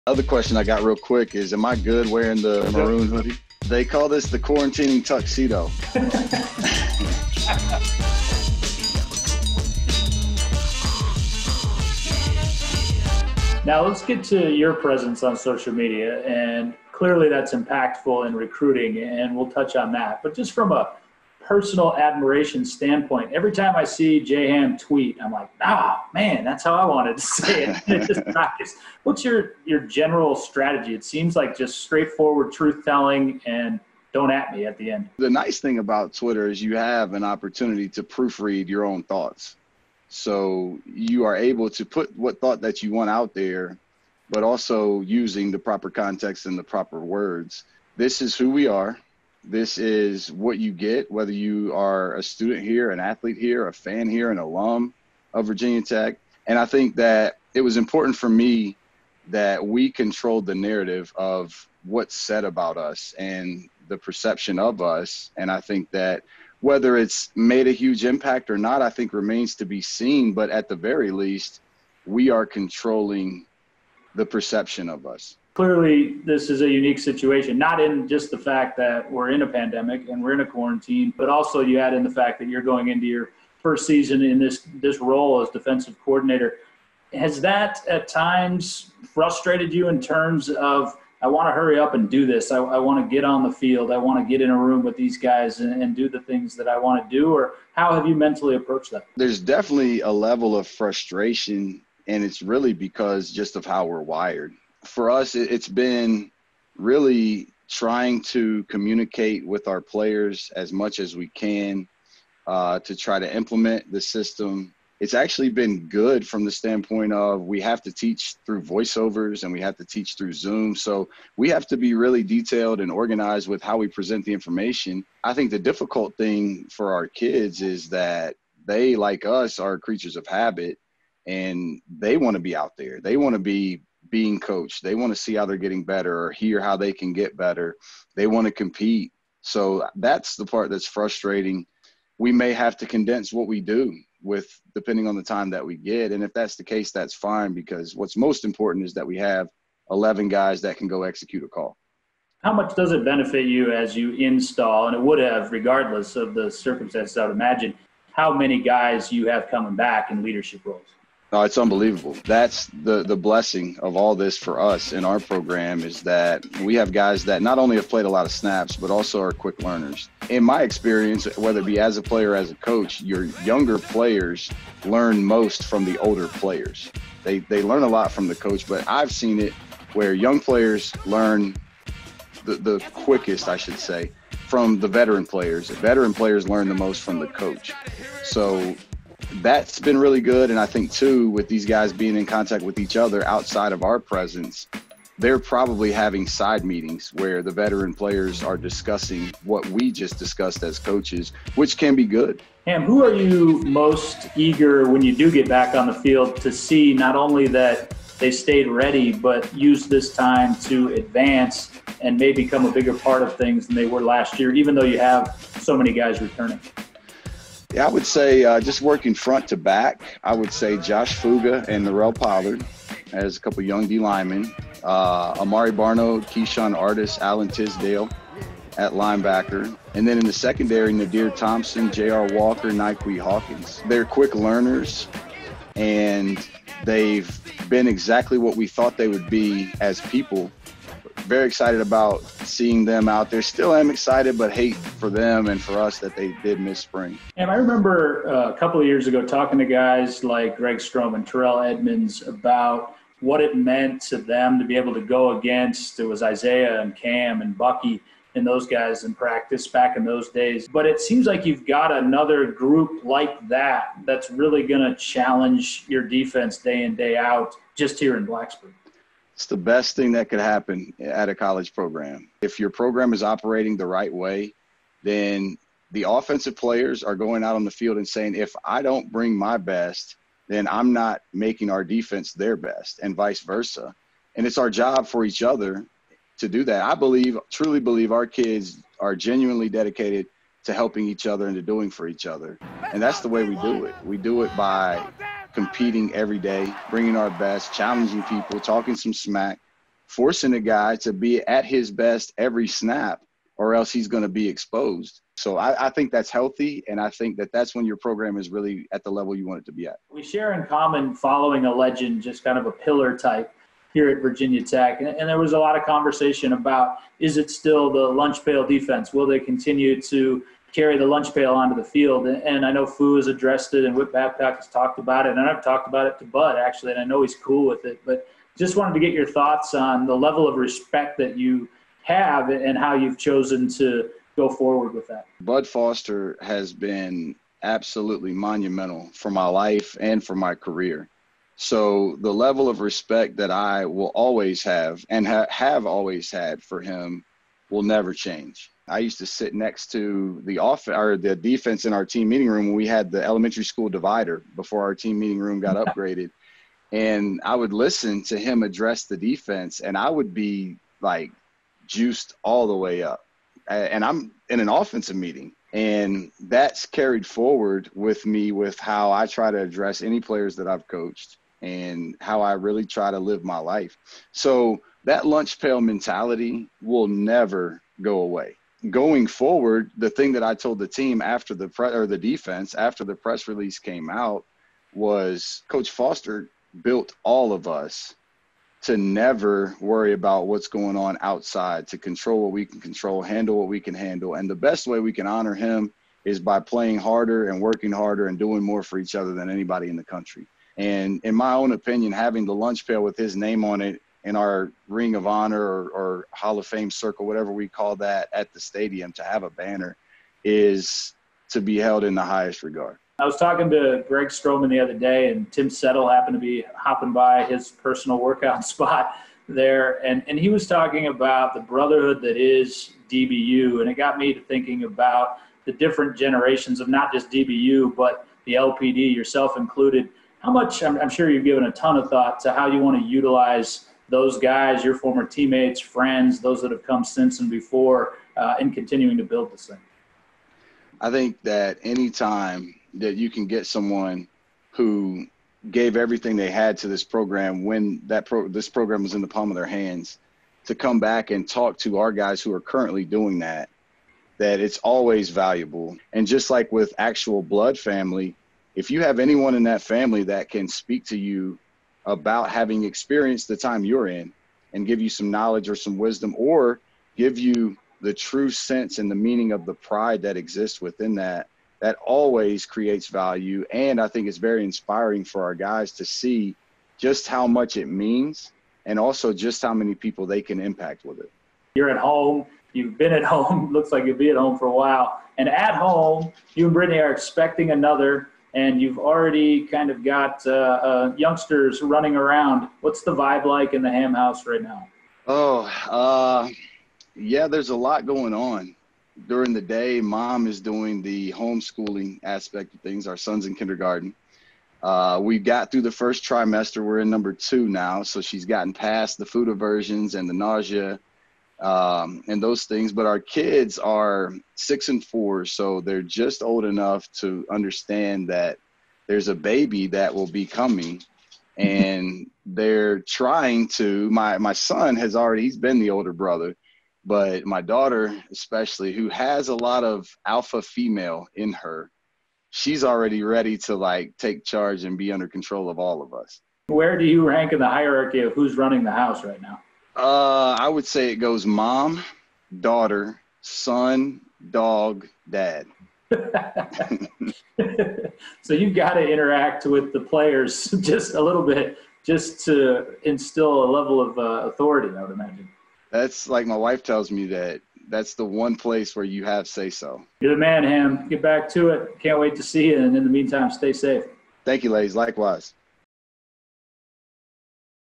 To the House feat. Justin Hamilton
Jon Laaser catches up with Hokies' Defensive Coordinator Justin Hamilton. The two discuss how the new defense is being installed in unconventional ways, impact returning players and the legacy of Bud Foster.